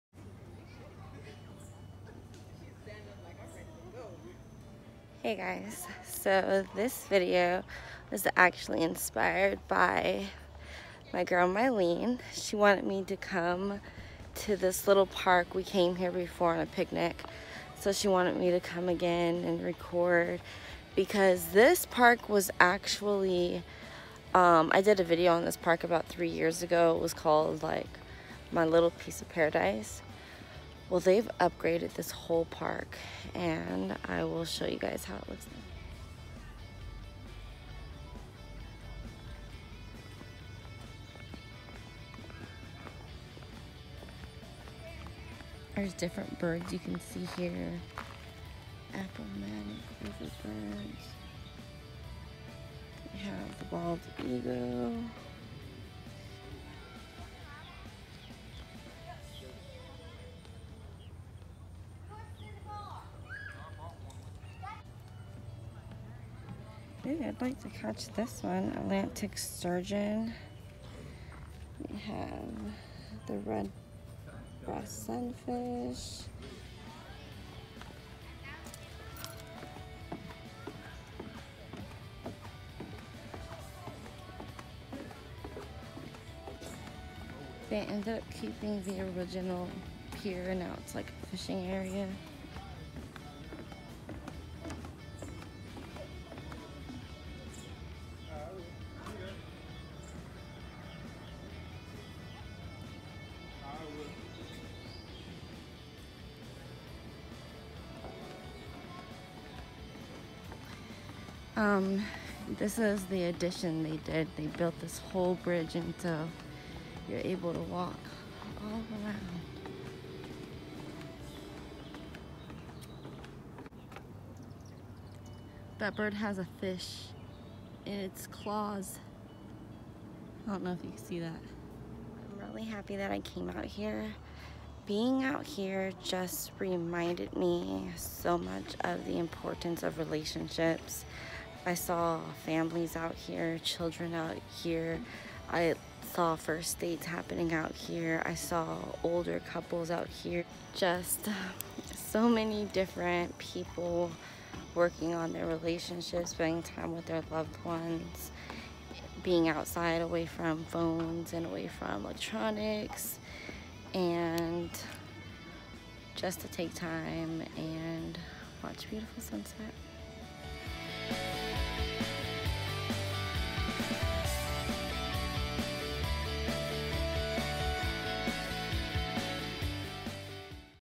Hopewell Riverwalk Sunset Tour
This park used to be very simple but they recently upgrade and it’s definitely improved the quality of community in this little city!